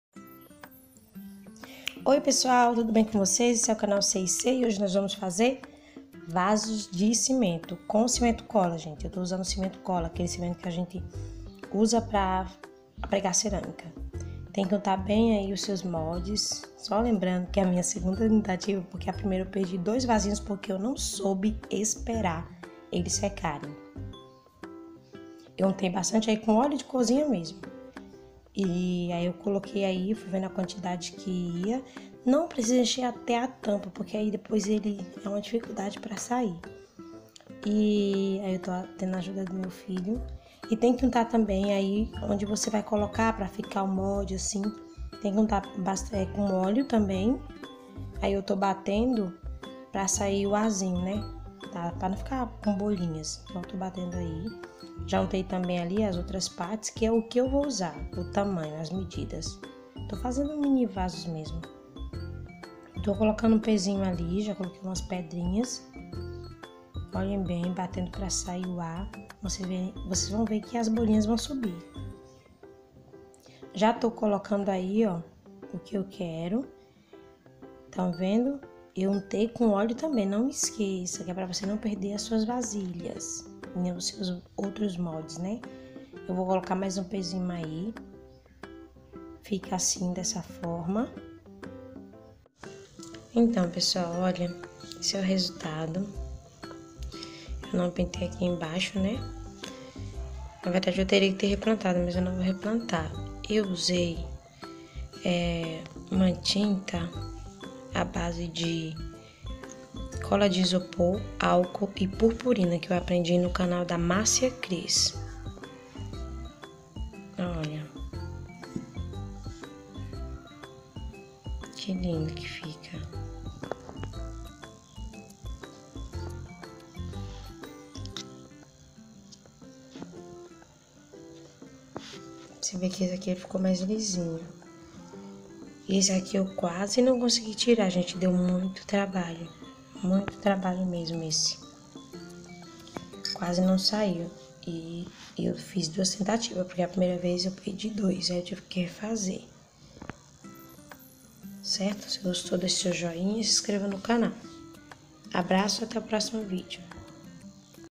Vasos de cimento (diy) by Maíra Santana
*A MASSA CORRIDA ELA DESMANCHA UM POUCO APÓS A PEÇA PRONTA ENTÃO FAÇA UMAS MISTURA DE CIMENTO E ÁGUA E PASSE NOS VASOS.
COM ESSA TINTA QUE EU FIZ AJUDOU A SELAR O GRAOZINHO DO CIMENTO.
ABRAÇOS...